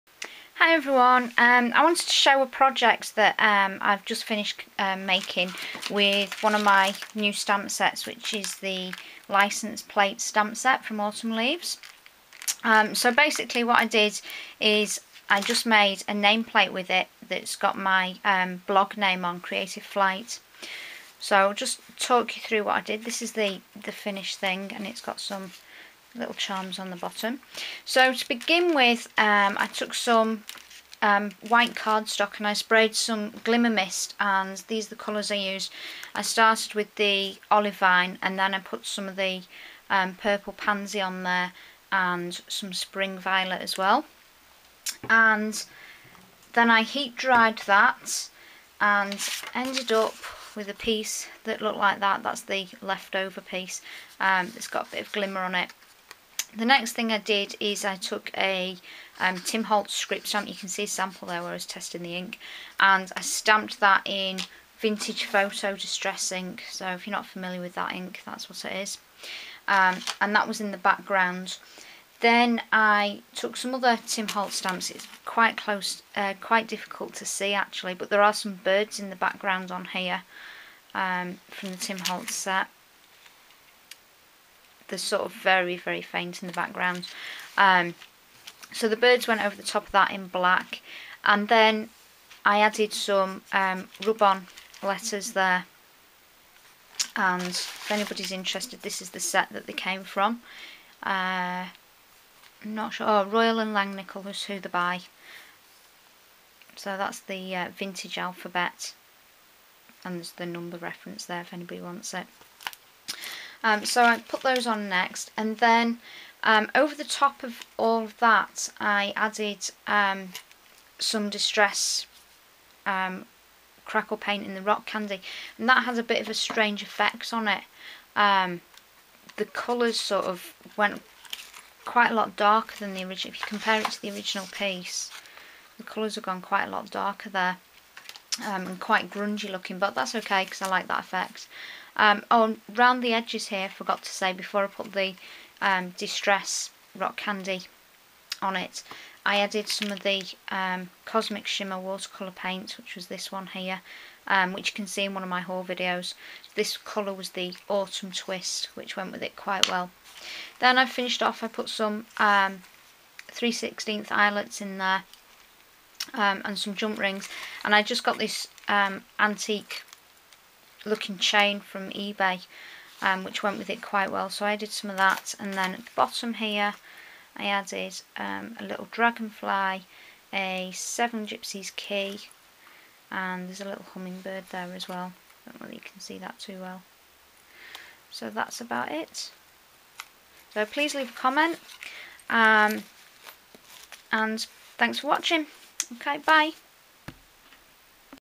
Mixed Media / Stamping Licence Plate Project
Name plate made using the Licence Plate stamp set by Autumn Leaves.